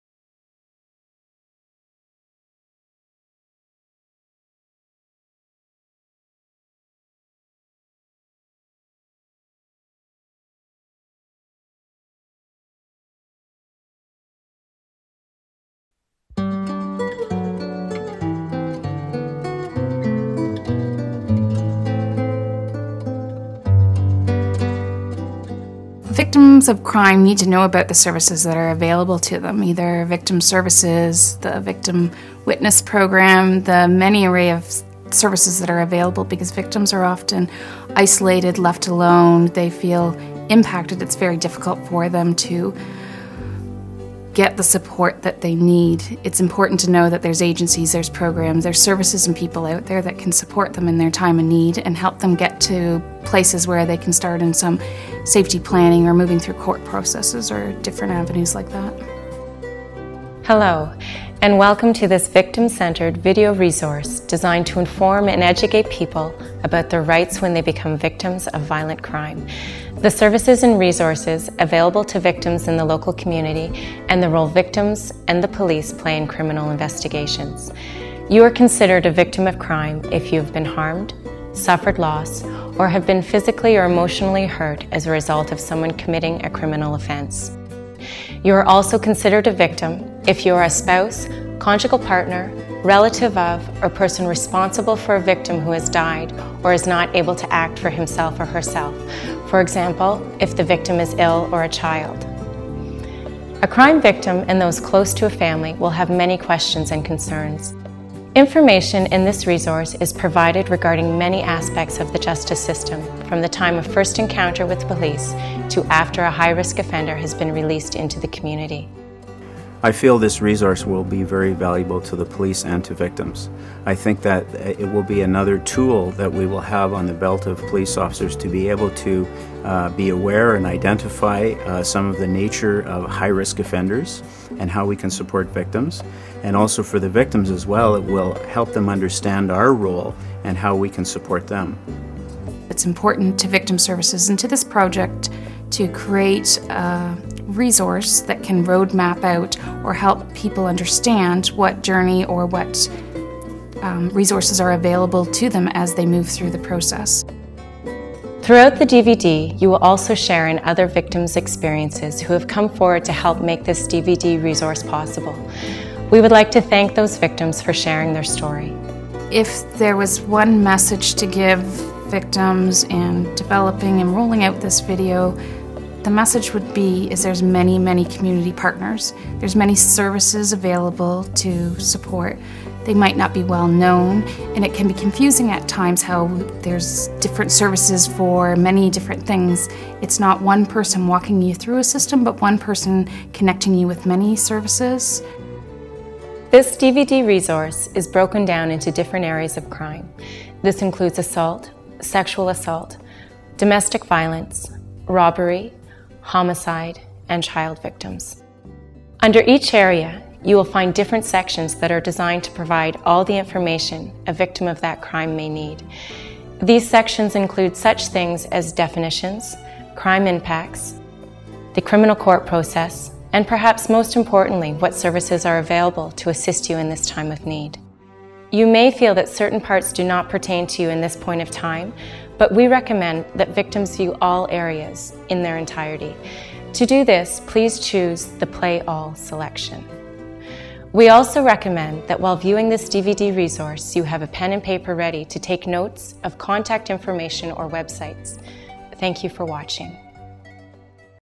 01-Introduction - Victim's Safety Guide
The Victim's Safety Guide video resource is a series of videos to inform people about their rights when they become victims of violent crime. You will also find information on services and resources available as well as the role victims and police play in criminal investigations.
with additional services donated by Pay It Forward Productions.